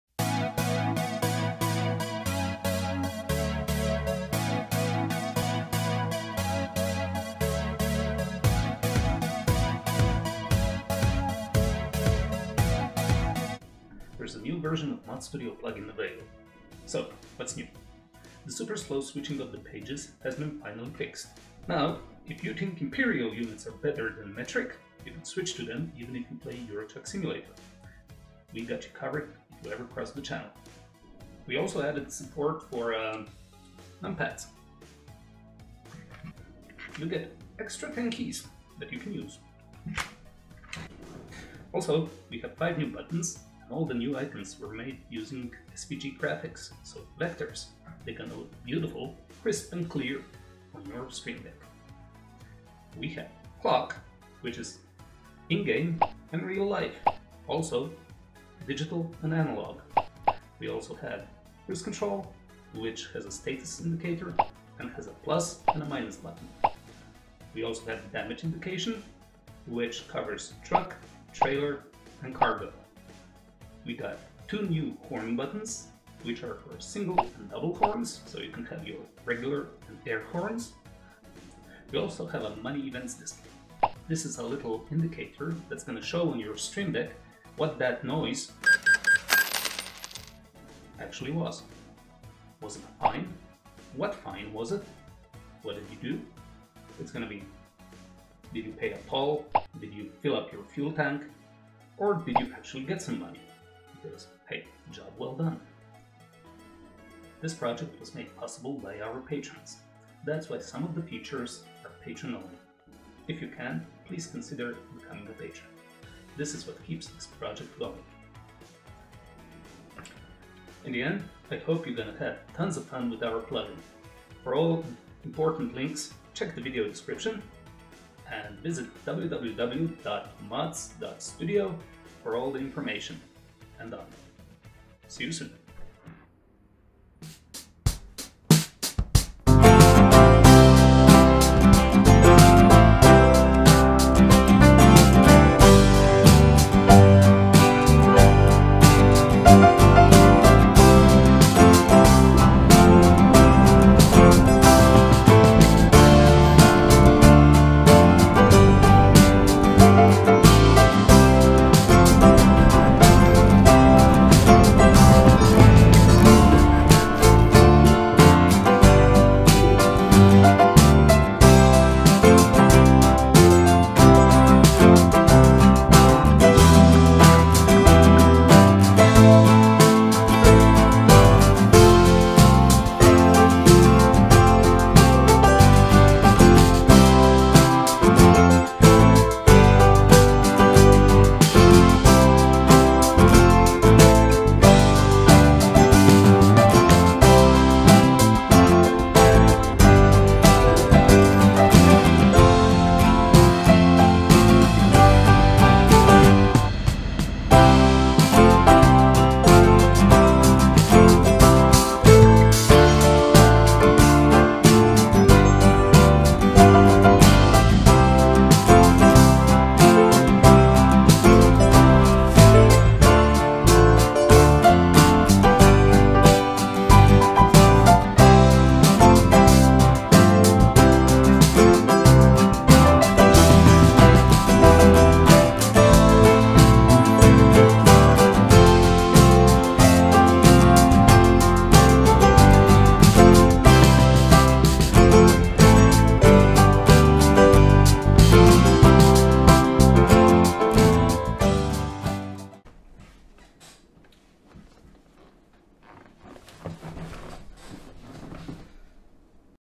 Mods Studio ETS2 and ATS Stream Deck Plugin just got an update! (v 3.0)
Hey, I'm still not friendly with the camera... but I'll get there... eventually. :)

Don't have a Stream Deck yet? Get your own here:
Stream Deck XL
Stream Deck 15-key
Stream Deck Mini

Chapters:
00:00 Intro
00:17 Bug fixes and enhancements
00:40 New buttons
01:46 Patreon support
02:00 Outro
02:16 Thank you to our Patrons!